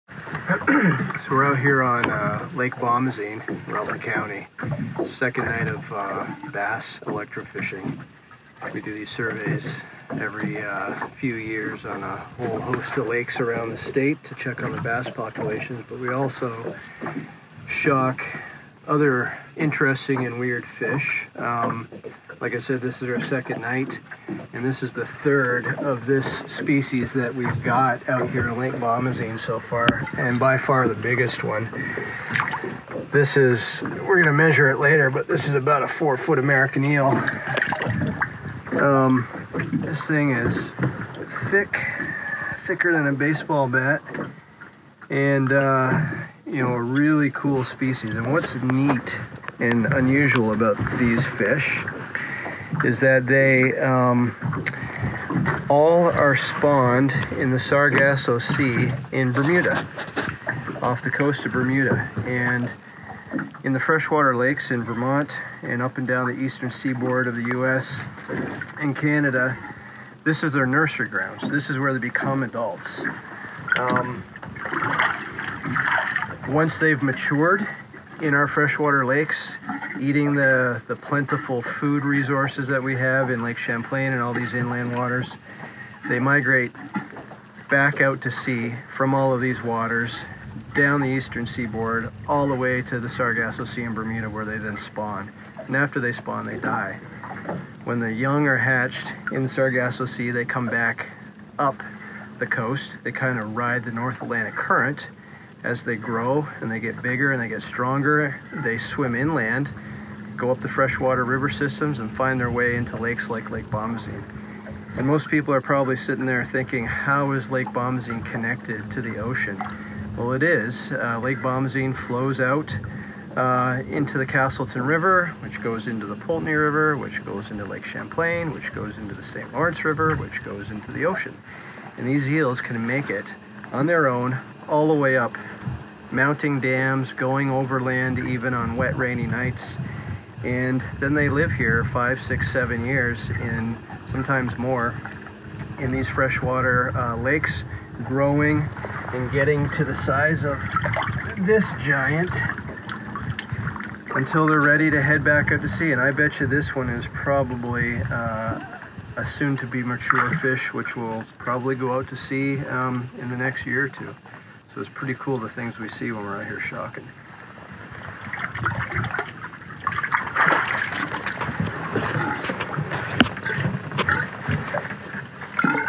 American Eel in Lake Bomoseen, Vermont. How did it get here!?
Did you know that there is a marine fish species that travels to Vermont from Bermuda, and then back again? Did you know that they spend their juvenile years in Lake Champlain as well as Vermont's freshwater inland lakes such as Lake Bomoseen, Lake Hortonia, and others?

Check out this little biology lesson on the American eel - a unique and wonderful native fish species with one of the most incredible migratory behaviors in the fish world!